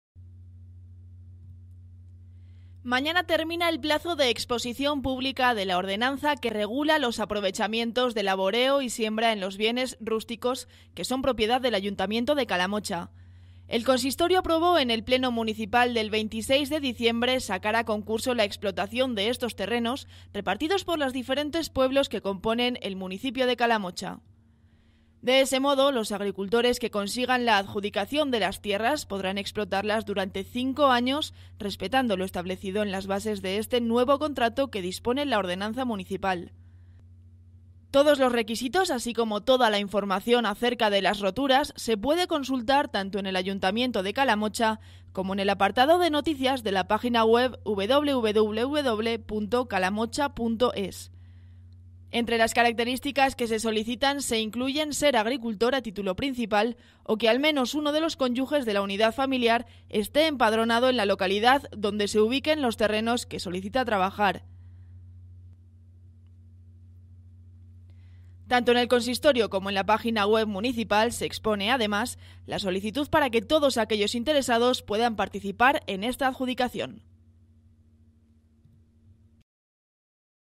19 de febrero, último día para consultar la ordenanza
El documento que recoge las condiciones acerca de la adjudicación de los terrenos municipales se expondrá en el Ayuntamiento y en la web hasta manaña para que los interesados puedan consultarlo y presentar las alegaciones correspondientes. También está expuesto el formulario de solicitud para los agricultores que quieran optar a la explotación de estas tierras.